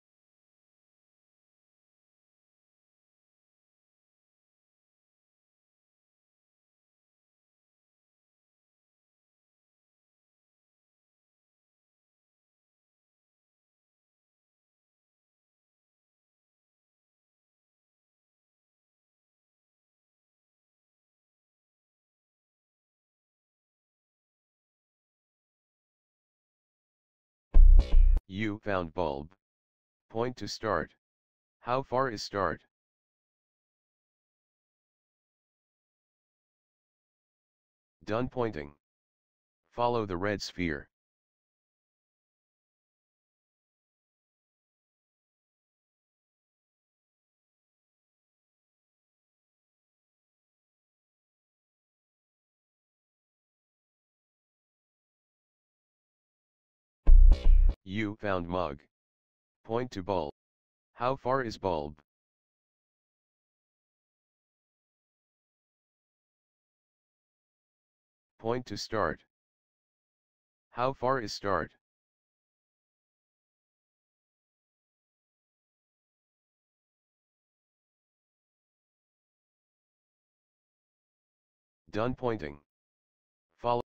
Illustration of spatial updating task in VR
Sample practice trial of the spatial updating experiment used in our study on investigating immersive virtual reality in architectural practice.
See Jake's thesis below for details:
Freiberg, J. (2015). Experience Before Construction: Immersive Virtual Reality Design Tools for Architectural Practice (MSc Thesis). Simon Fraser University, Surrey, BC, Canada.